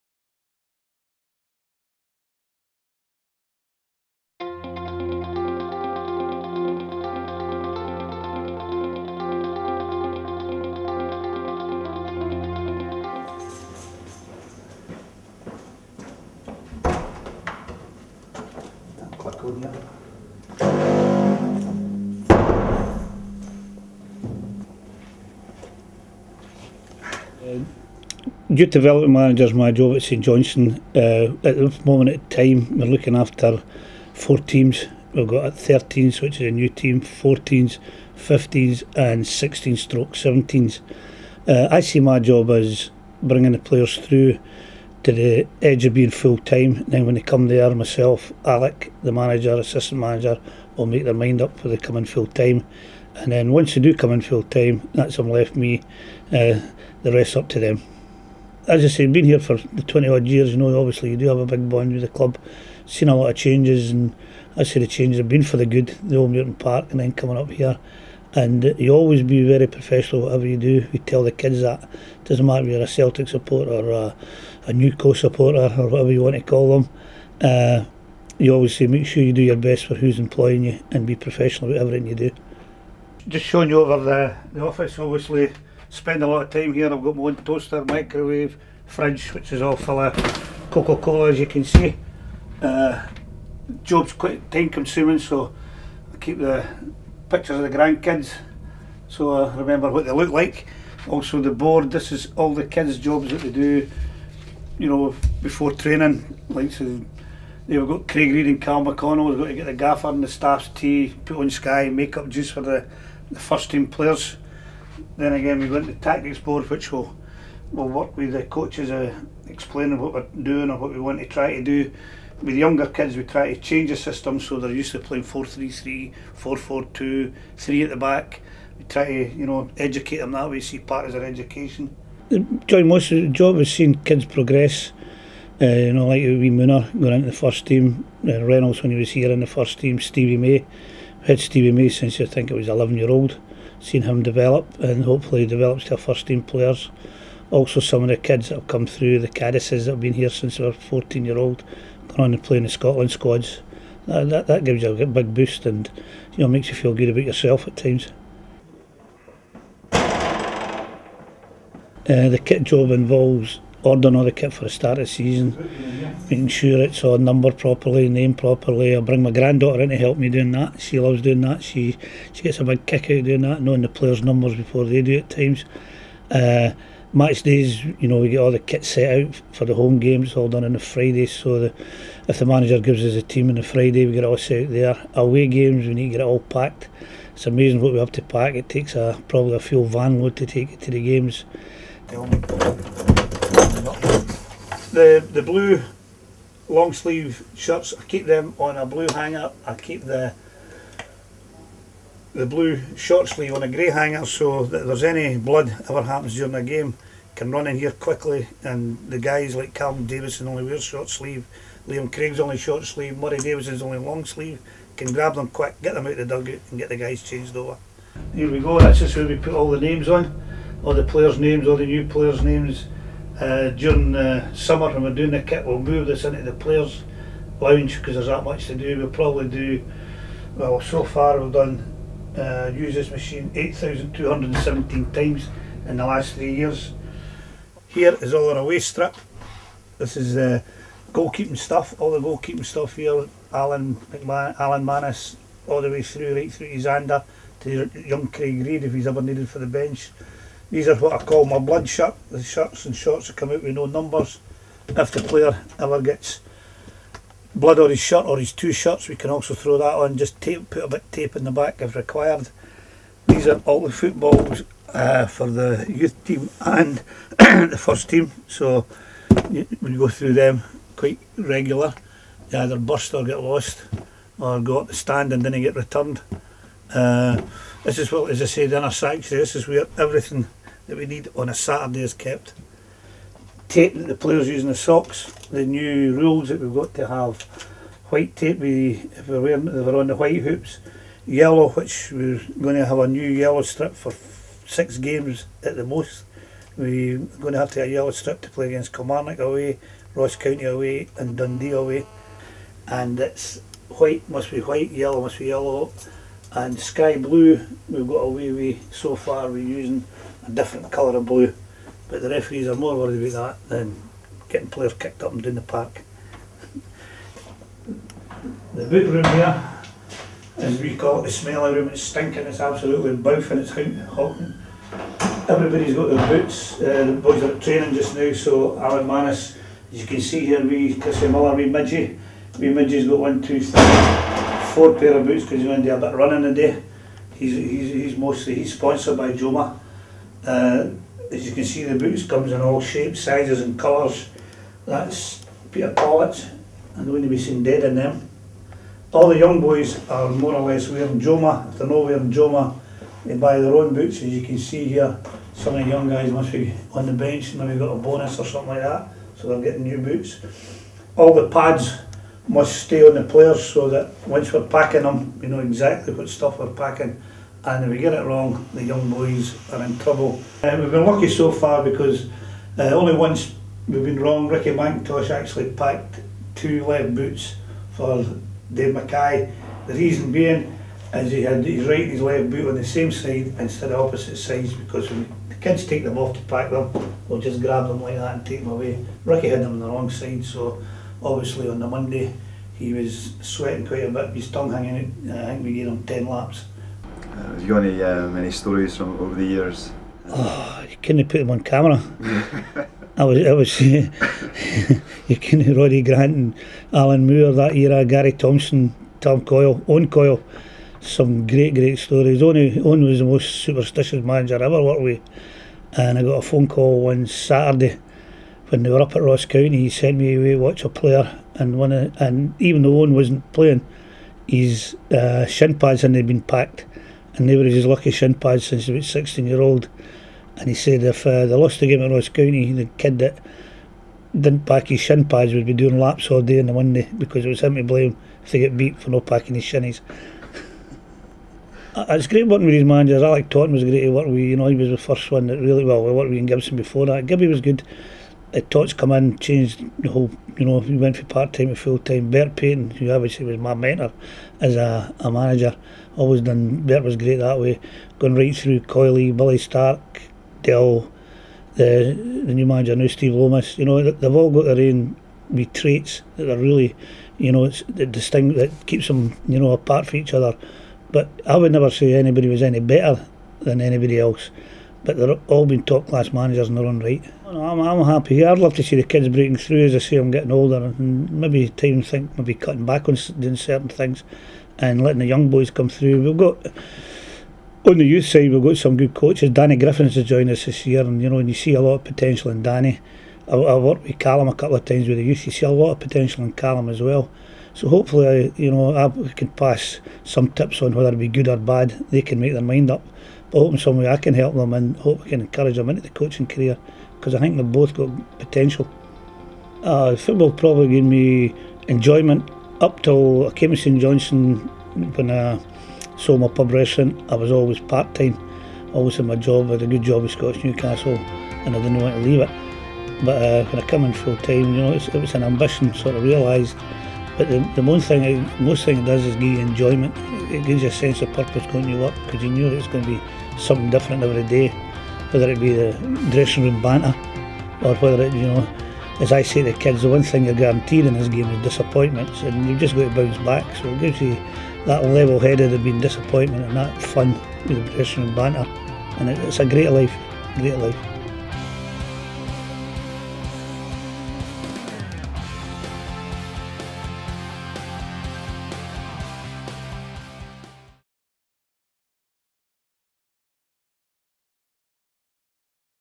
Tommy Campbell feature
SAINTS TV goes behind the scenes to speak to Youth Development Manager and Kitman Tommy Campbell on his role at the club.